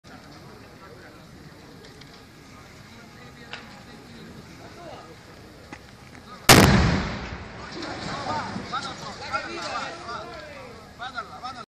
L&#39;incendio a Vinci
Camion e capannoni in fiamme